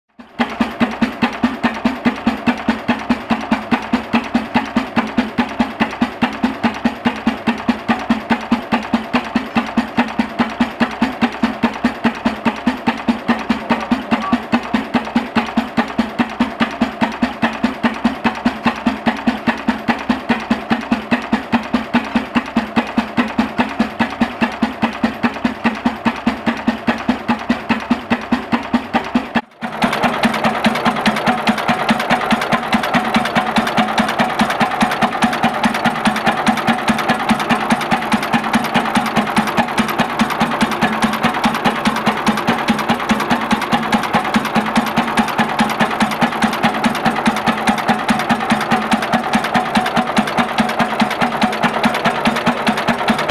Avery 12-25
Edwin was a die hard Rumely Oil Pull man!  This is an old collection and nearly a complete lineup of Oil Pulls as well as a great Case Steam Engine, a little 12-25 Avery and a rare Oil-Turn Rumely Power Unit.  Don't forget the Corvette!